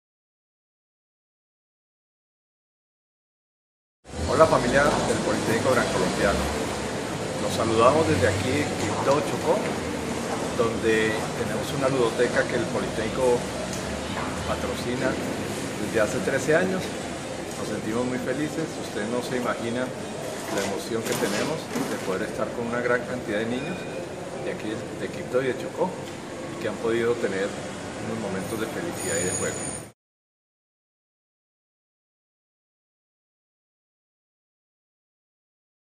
El Poli hace presencia en el departamento del Chocó
Nuestro presidente Fernando Dávila Ladrón de Guevara, habla acerca de la responsabilidad social de la institución con la ludoteca de Quibdó NAVES “Cocorobé